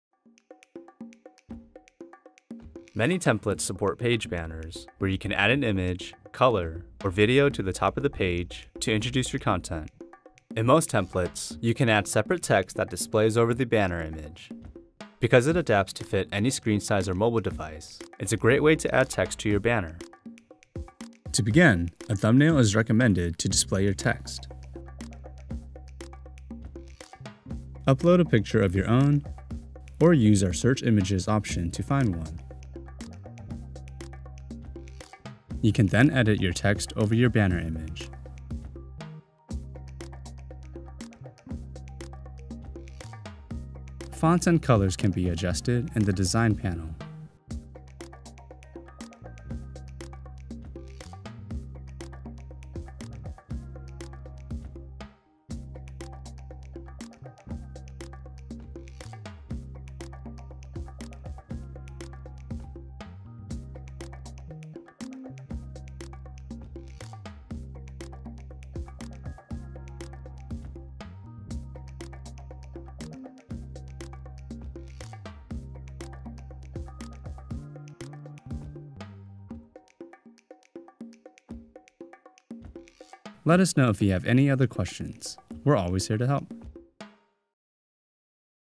How to Add Images and Text to Banners | Squarespace 7.1 (Classic Editor)
As you build your website and brand with Squarespace, add a banner image that displays near the top of the page, which adds a visual element that sets the tone for your site. This video goes through the steps for adding and styling banner images.